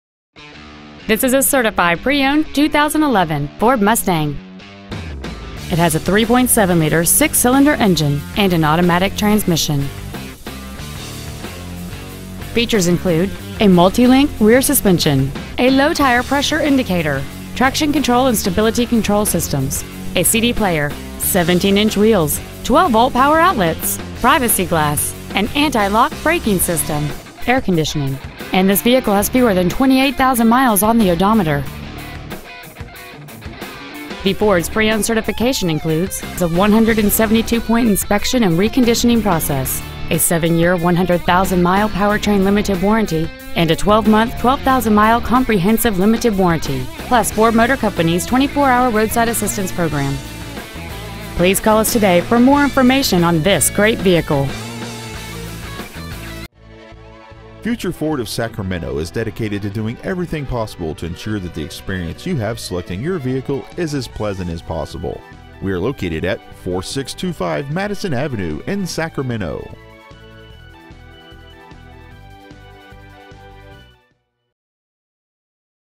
Used 2011 FORD MUSTANG Sacramento CA
Year : 2011
 Make : FORD
 Model : MUSTANG
 Engine : 3.7L V6 24V MPFI DOHC
 Trans . : AUTOMATIC
 Exterior : OTHER
 Miles : 27,402

 Stock : P83584